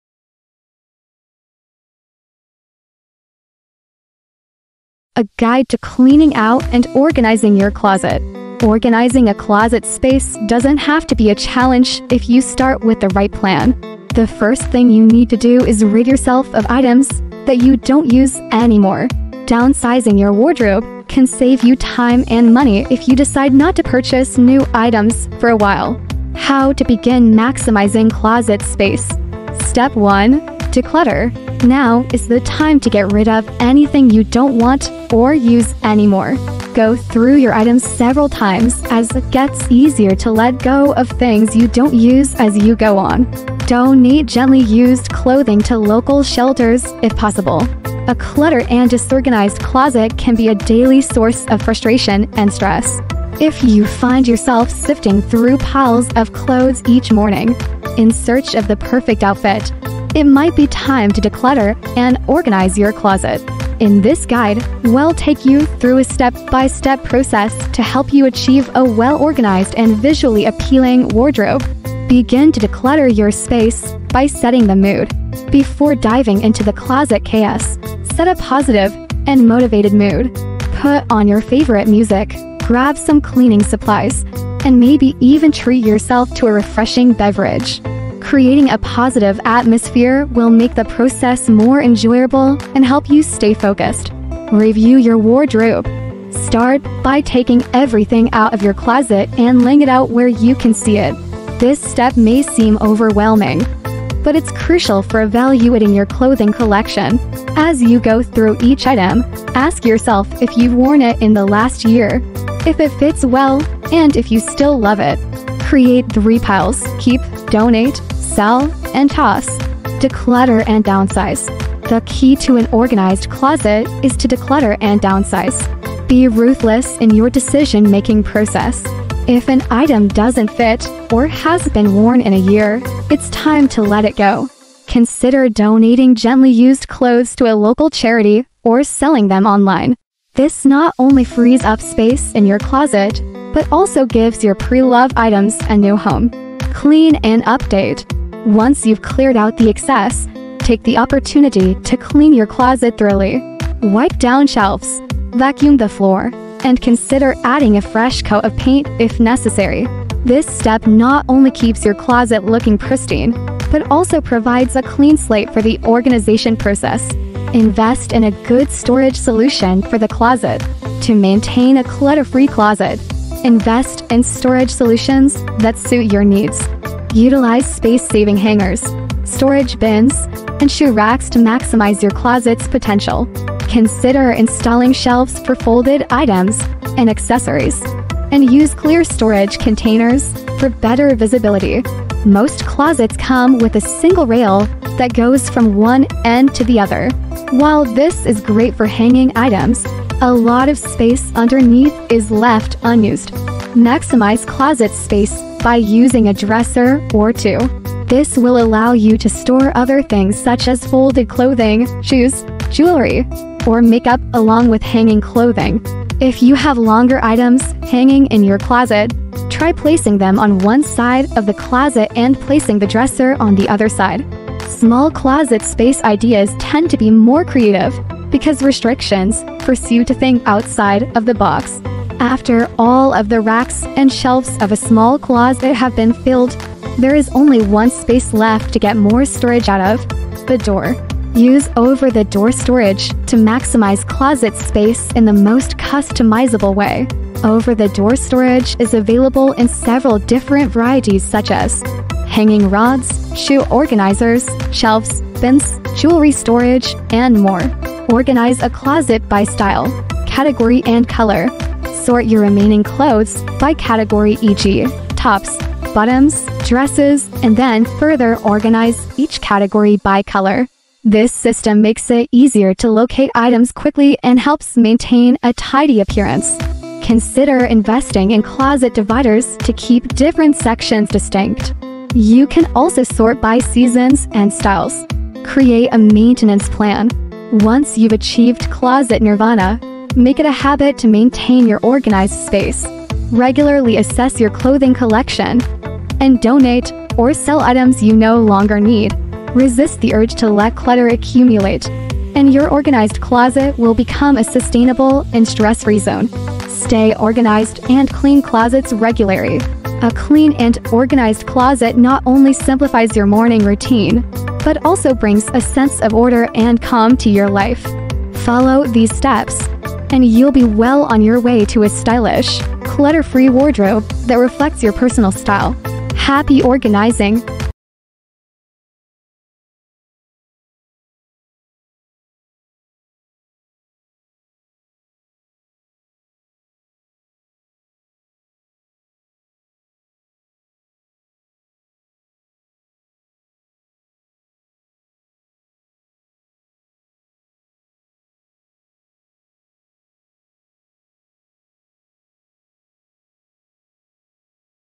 A Step-by-Step Guide to Cleaning Out and Organizing Your Closet. How to Maintain a Clean Closet
A cluttered and disorganized closet can be a daily source of frustration and stress. If you find yourself sifting through piles of clothes each morning in search of the perfect outfit, it might be time to declutter and organize your closet. In this guide, we’ll take you through a step-by-step process to help you achieve a well-organized and visually appealing wardrobe.

Begin to Declutter Your Space by Setting the Mood
Before diving into the closet chaos, set a positive and motivated mood. Put on your favorite music, grab some cleaning supplies, and maybe even treat yourself to a refreshing beverage. Creating a positive atmosphere will make the process more enjoyable and help you stay focused.

Closet Organizers and Storage

Helpful Crochet Books